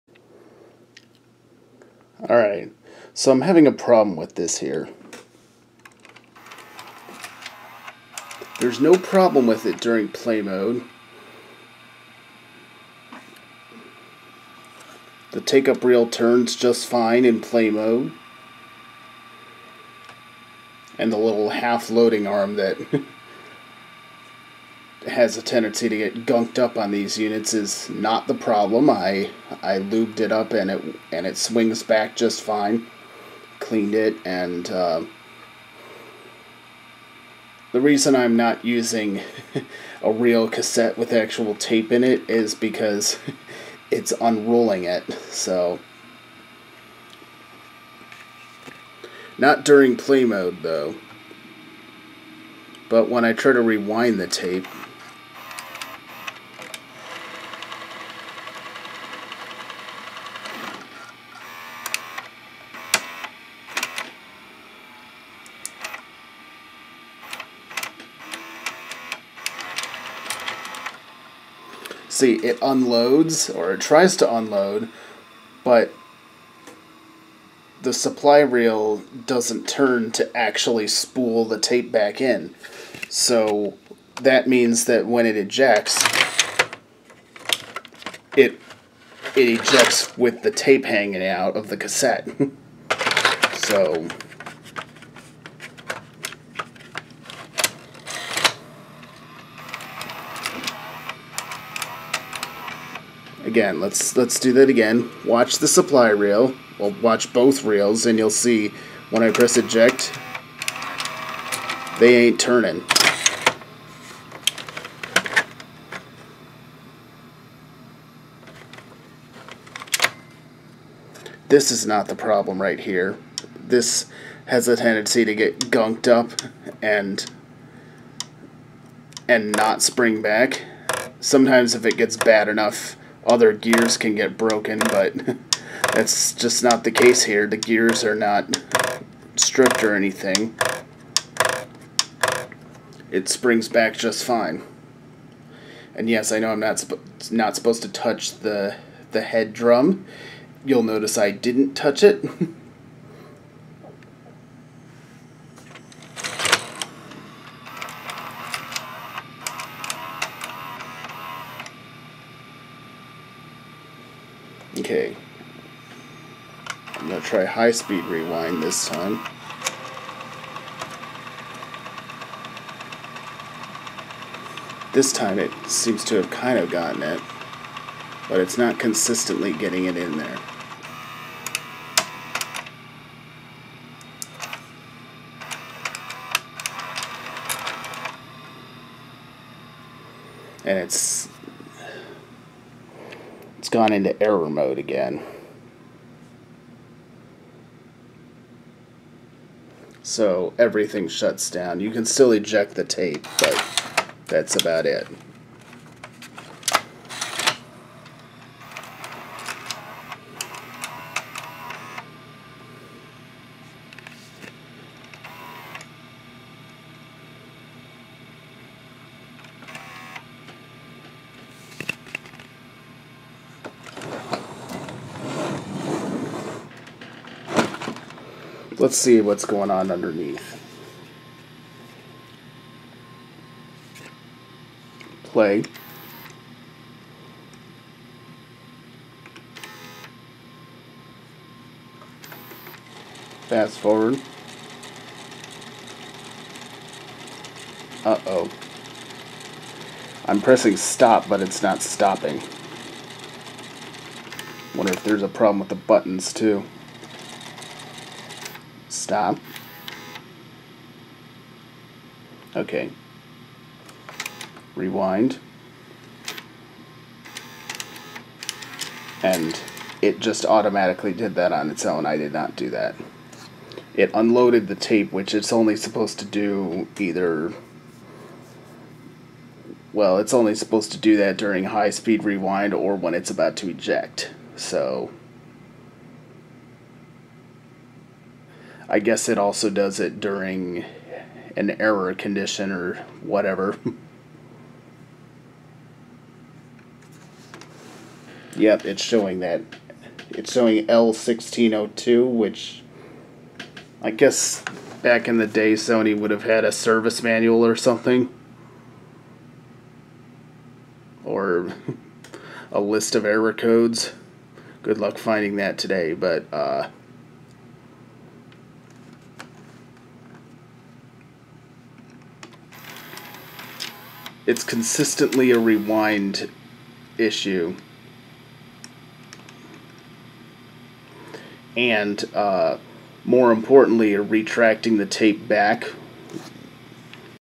Sony VCR SLV-595HF problems: Reels not turning during eject
The reels don't turn during eject mode, so the tape gets caught inside when you try to eject the cassette. It's a nice design in terms of repairability so I'd rather not just throw it away. Was hoping someone could point me in the right direction in terms of a way to repair the unit.